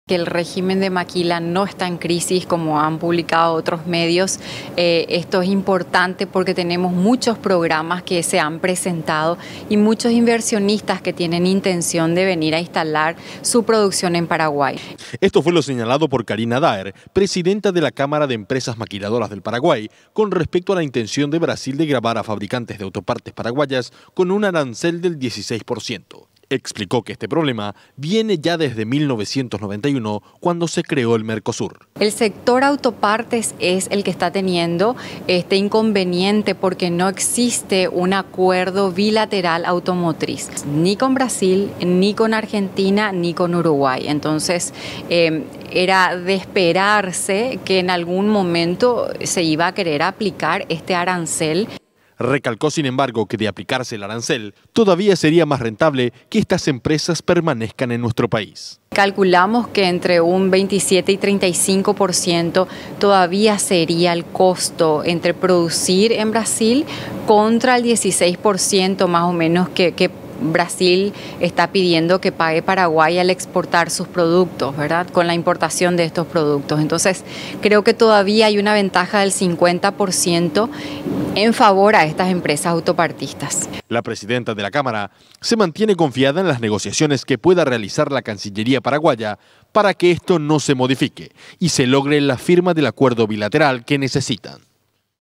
Hay buena expectativa de Maquiladoras
La Cámara de Empresas Maquiladoras del Paraguay ve con buenos ojos la negociación iniciada por la Cancillería paraguaya con Itamaratí para solucionar el problema surgido en el rubro de las autopartes con la intención del Brasil de cobrar aranceles a las empresas paraguayas.